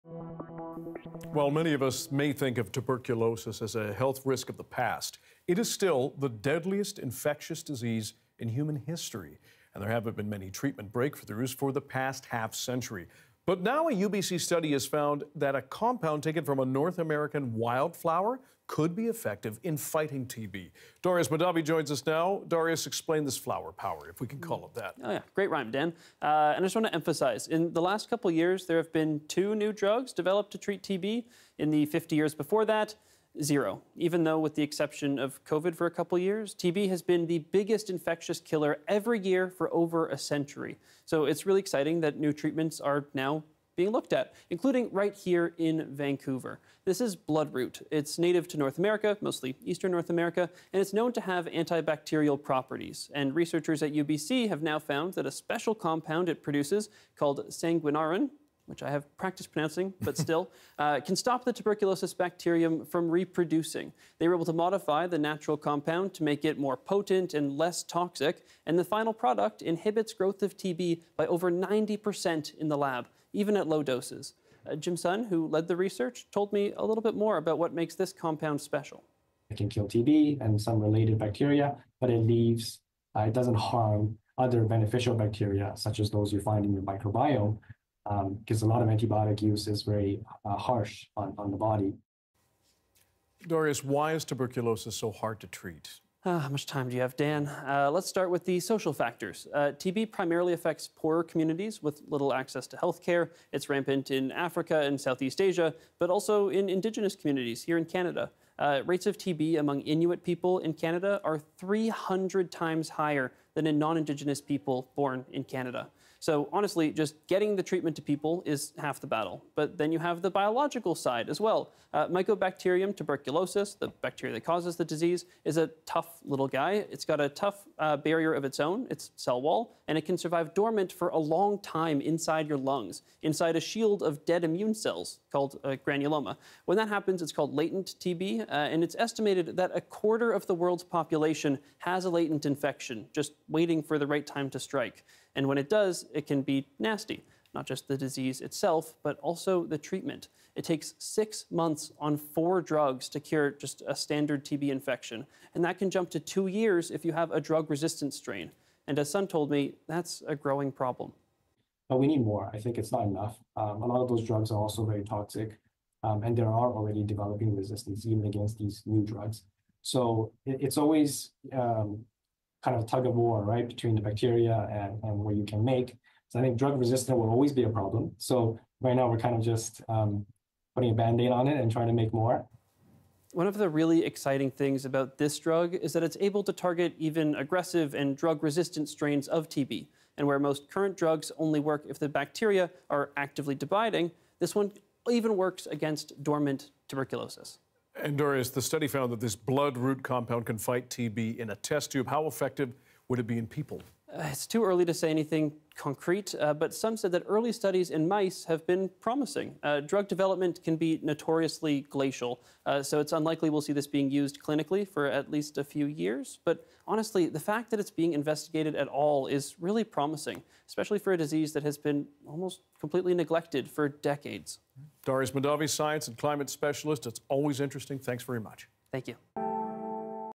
Wildflower compound may help fight tuberculosis, research finds
The deadliest disease in human history, few treatment breakthroughs have been made on tuberculosis in recent decades. Now, researchers out of UBC believe they've found a possible answer within a North American wildflower. CBC’s Climate Communicator Darius Mahdavi explains more.